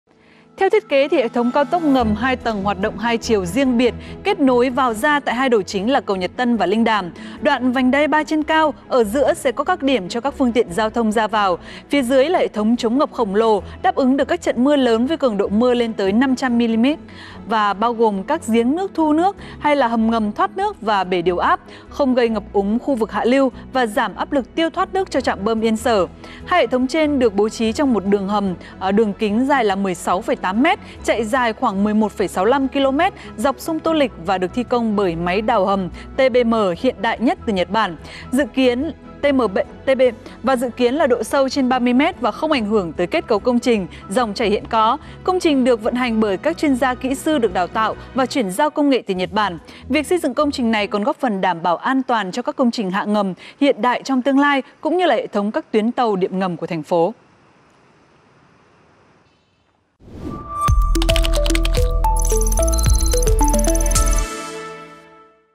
Đề xuất xây dựng hầm chống ngập kết hợp cao tốc dưới lòng sông Tô Lịch | VTV24
Công ty Cổ phần Tập đoàn Môi trường Nhật Việt (JVE) đã đề xuất dự án xây dựng hầm chống ngập kết hợp cao tốc dưới lòng sông Tô Lịch. Đây là đơn vị đã thí điểm thành công việc làm sạch sông Tô Lịch và khử mùi bãi rác Nam Sơn thời gian vừa qua. Dự án nếu được chấp thuận dự kiến sẽ xây dựng trong 5 năm, kết hợp với tổng thầu xây dựng lớn nhất Nhật Bản.